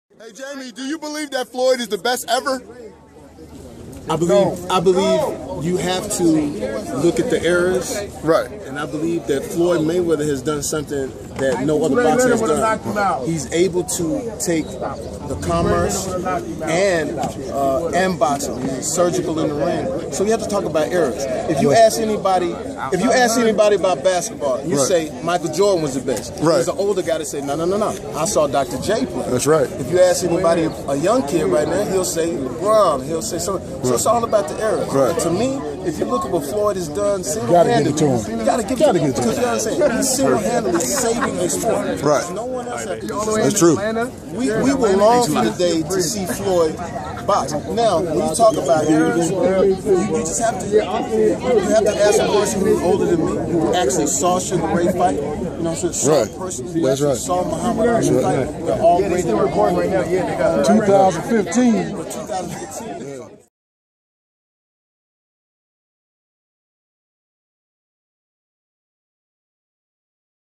JAMIE FOXX PRAISES FLOYD MAYWEATHER; SAYS FANS WILL MISS HIM WHEN HE RETIRES
"I believe you have to look at the eras...Floyd Mayweather has done something that no other boxer has done...he's single-handedly saving a sport...we will long for the day to see Floyd box," stated Academy Award winner Jamie Foxx, who had high praise for undefeated pound-for-pound king Floyd Mayweather. Check it out!